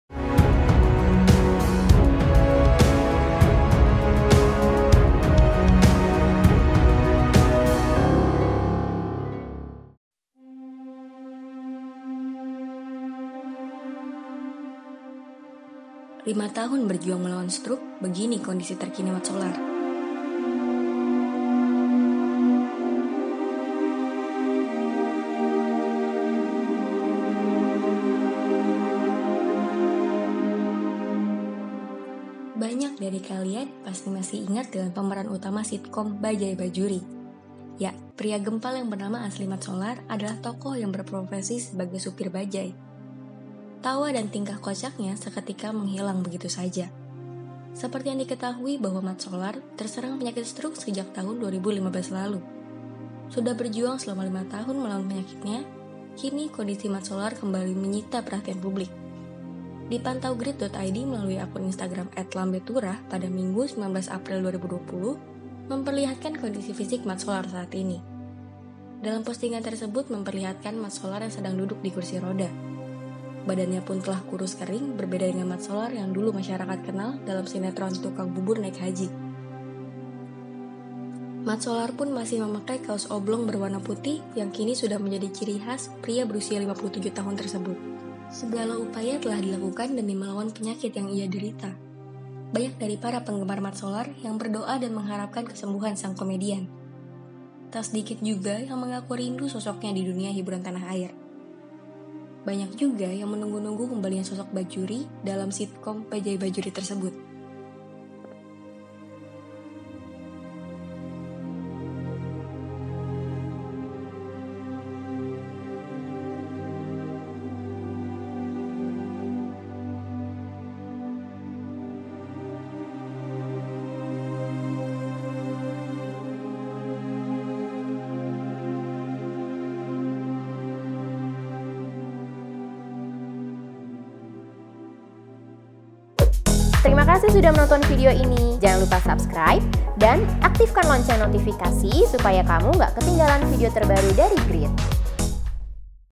Sudah Lama Tak Terlihat Di Layar Kaca Indonesia, Begini Kabar Terkini Mat Solar
Grid.ID - Kalian pasti masih ingat dengan pemeran utama sitkom Bajaj Bajuri. Ya, pria gempal bernama asli Mat Solar adalah tokoh yang berprofesi sebagai supir bajaj.

Tawa dan tingkah kocaknya seketika menghilang begitu saja.

(PM)

EDITORIAL OFFICE

ADVERTISING